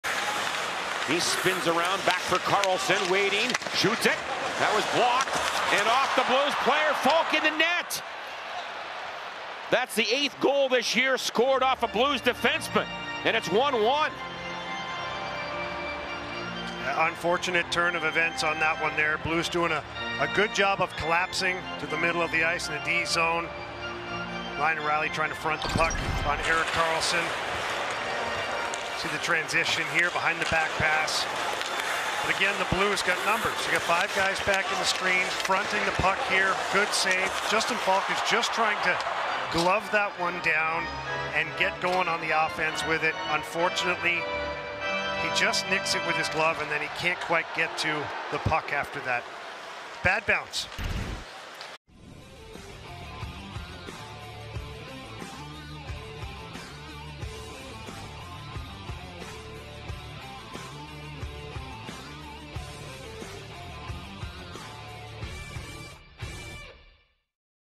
Justin Faulk Gifts Sharks A Goal By Gloving Puck Into His Own Net
Watch as St. Louis Blues defenceman Justin Faulk attempts to control the puck but ends up batting it into his own net after losing control of it with the glove.